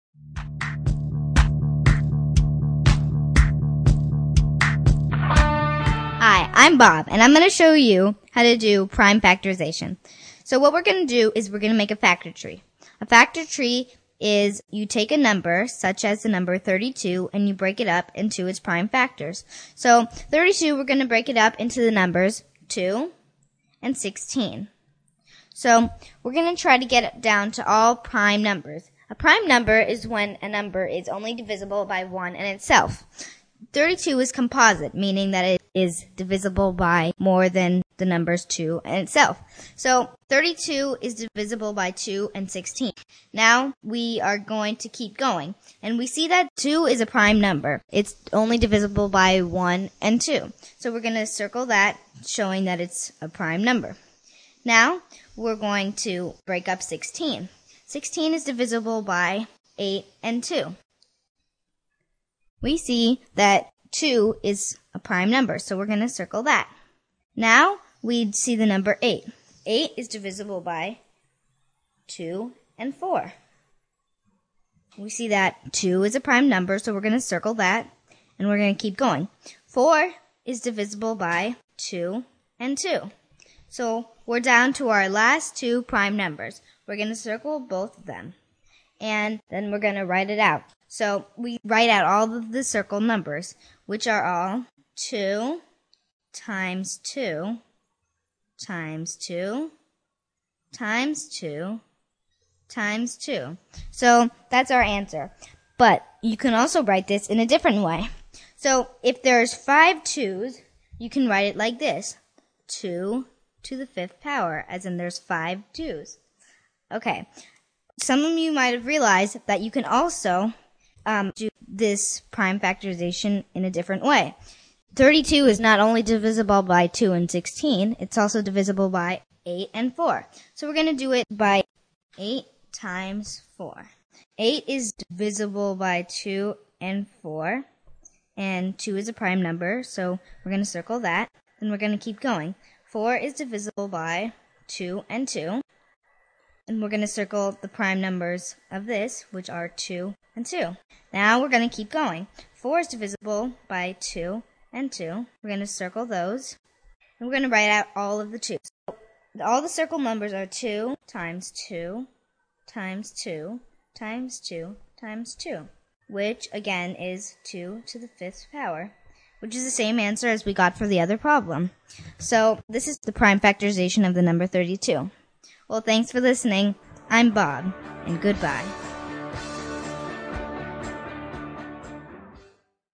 Prime Factorization (factor tree)
Sixth-grade student, "Bob", explains and shows how to make a factor tree and shows us prime factorization.